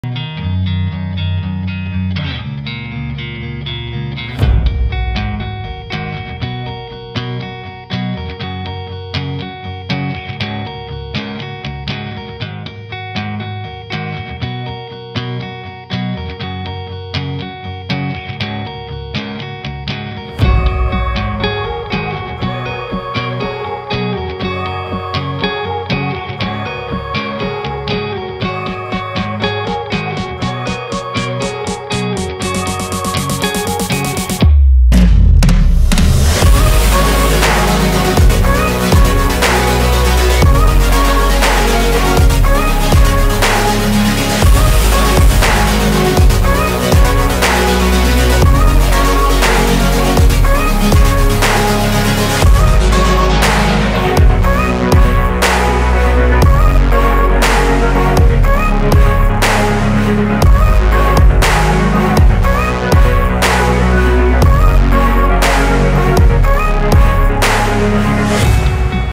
Container Ship 9400 TEU is berthing in Ningbo/China.Timelapse.
container